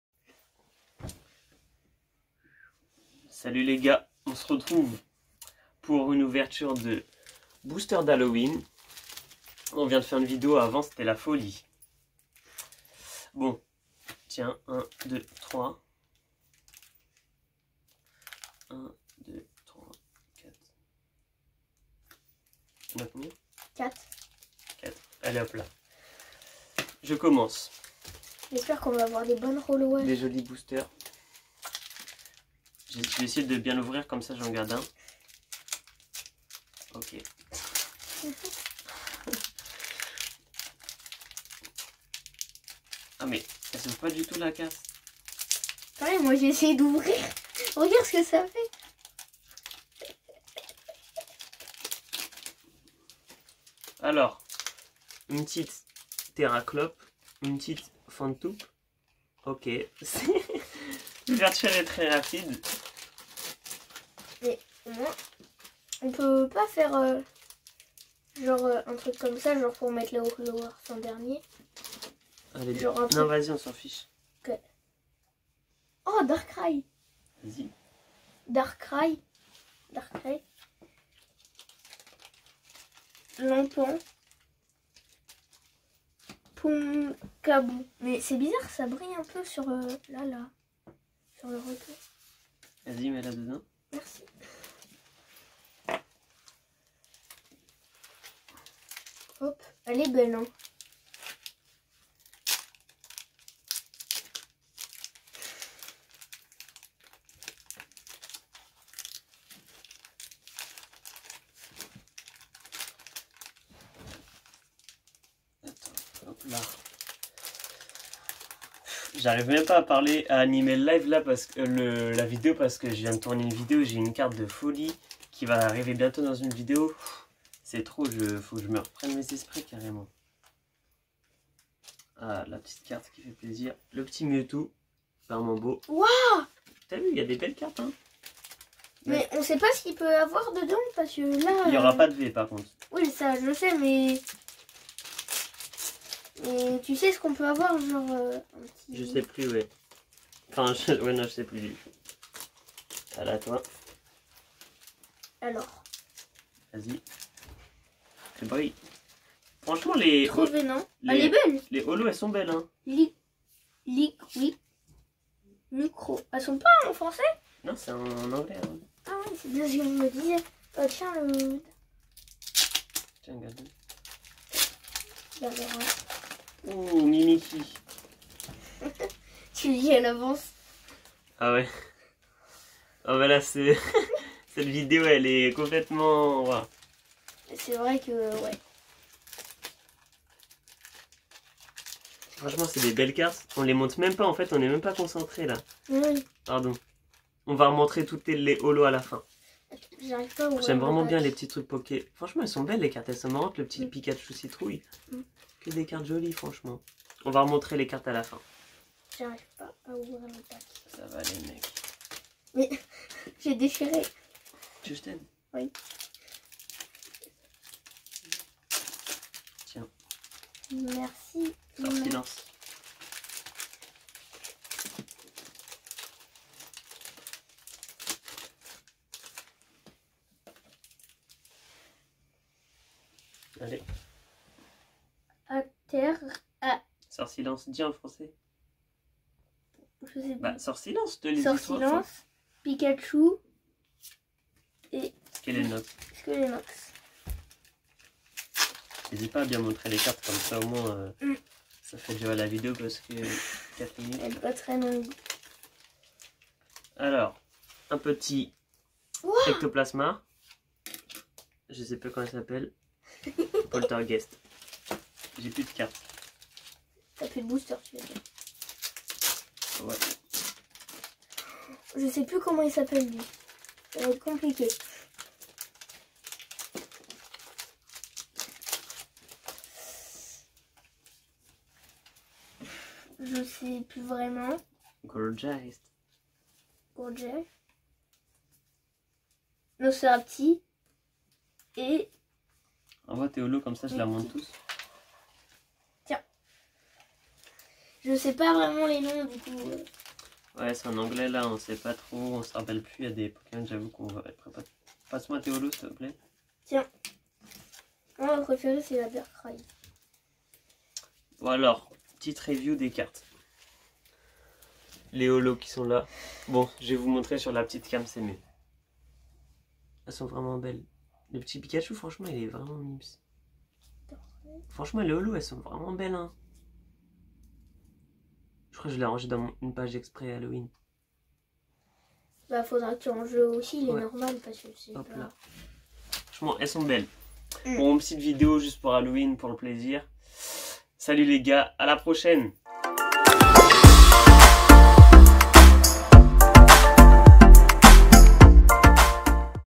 OUVERTURE DE BOOSTER D'HALLOWEEN AVEC AARON ( en retard LOL)
Salut les gars petite ouverture sympa pour halloween en anglais j'espère que vous allez apprécié :)